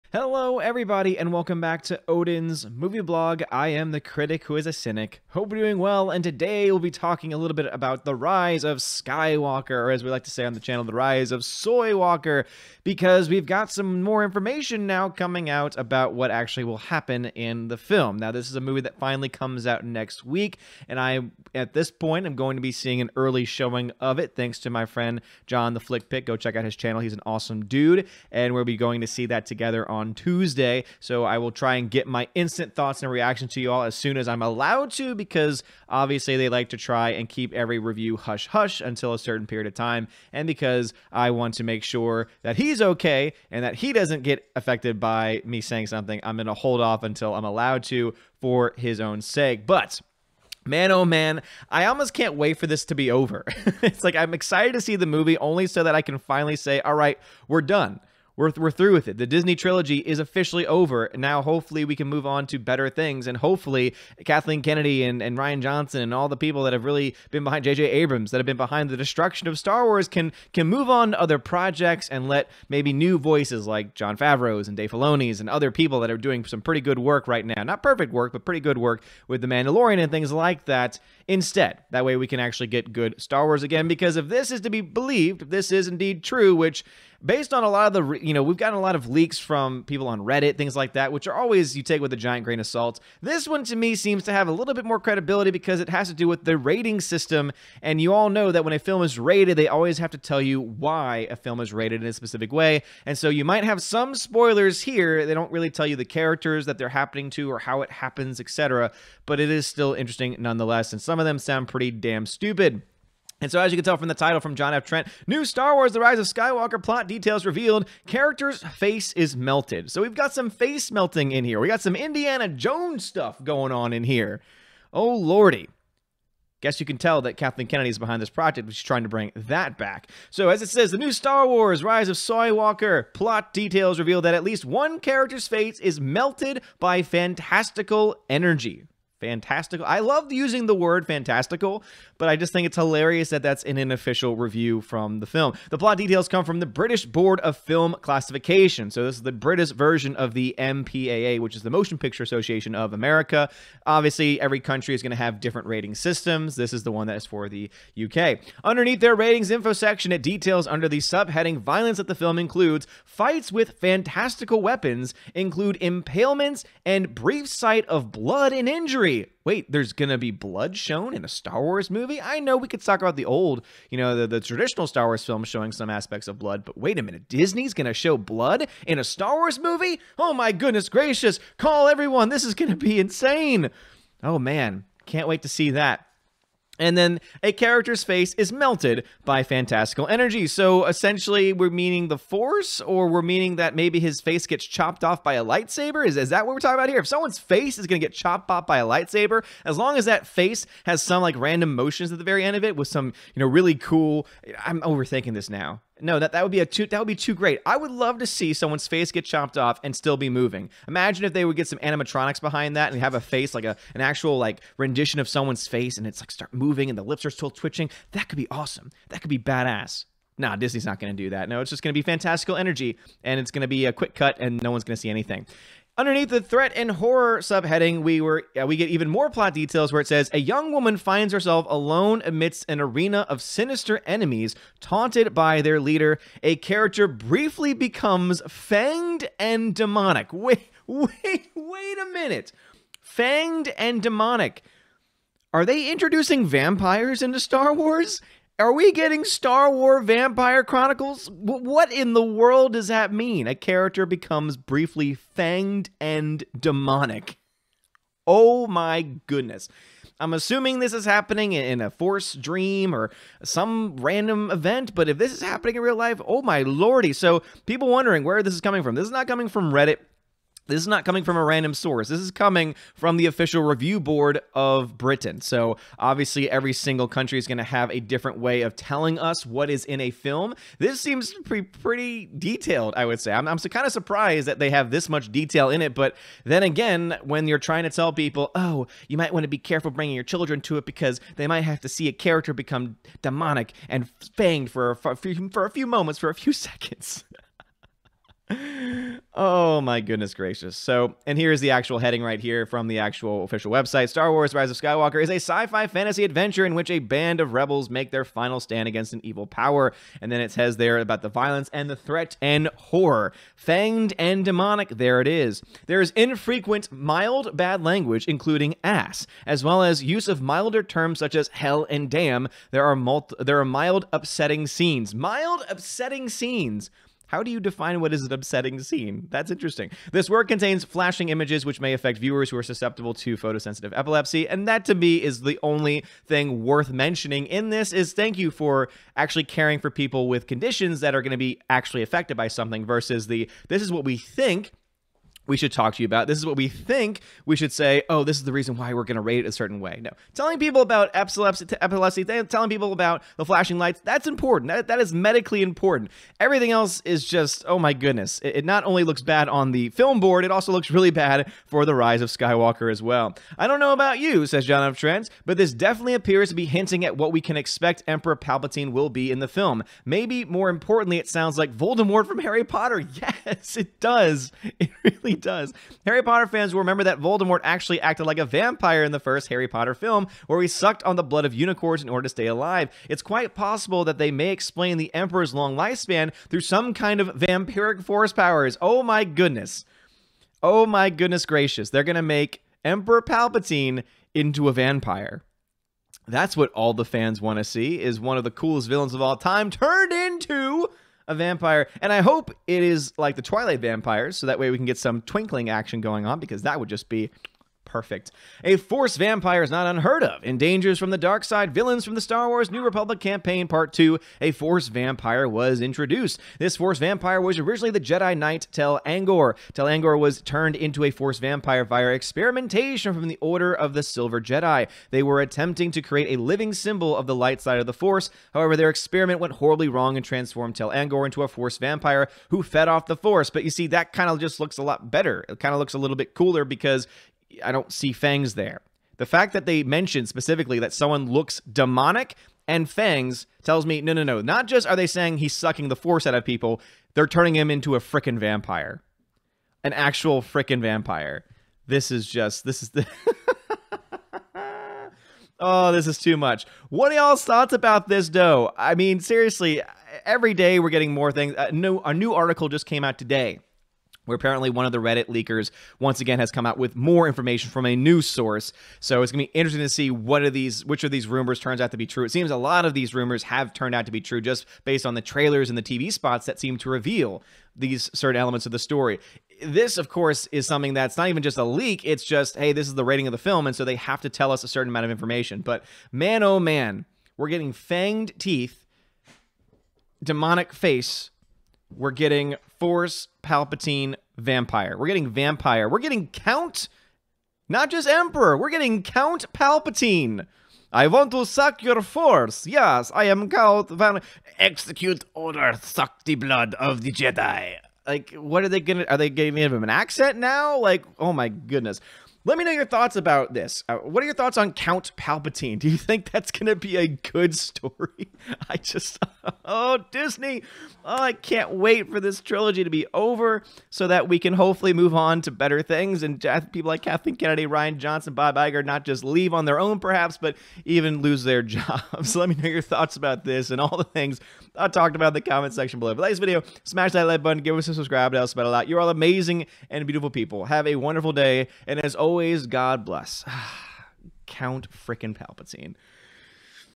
British Film Rating Suggests Vampire Palpatine for Rise of Skywalker
New leaks suggest that Palpatine will be a force vampire with fangs and all...yes...we are getting Count Palpatine...Episode IX will be a trainwreck